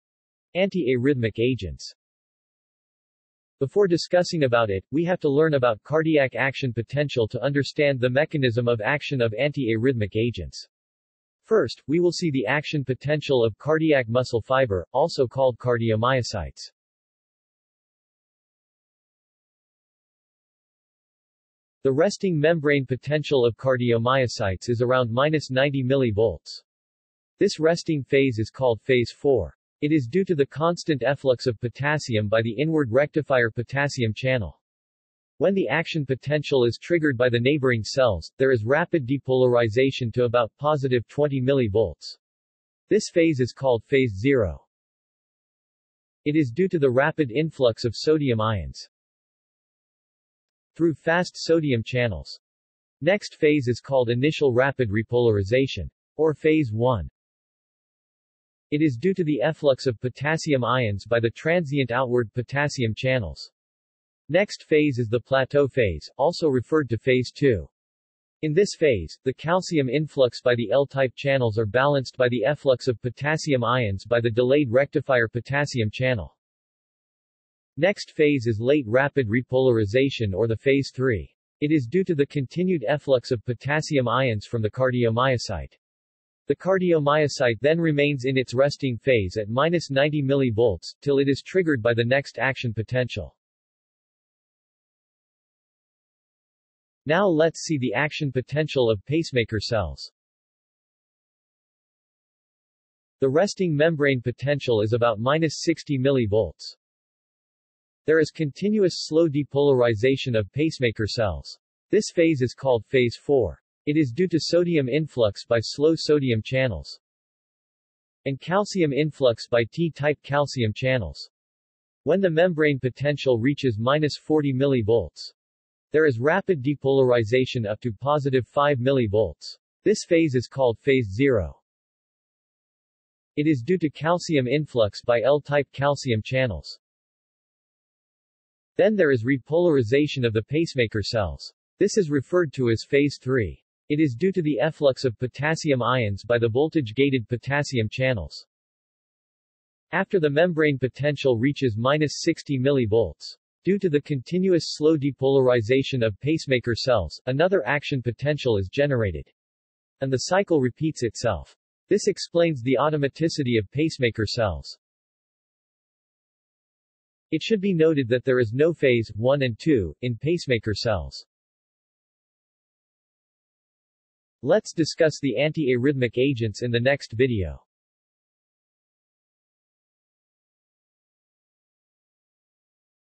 Antiarrhythmic drugs/ agents | Chapter 1: Cardiac Action Potential (Made Easy)
This video explains about the cardiac action potential in cardiomyocytes and pacemaker cells (Sinoatrial Node).
This is chapter 1 of the video explaining about antiarrhythmic agents.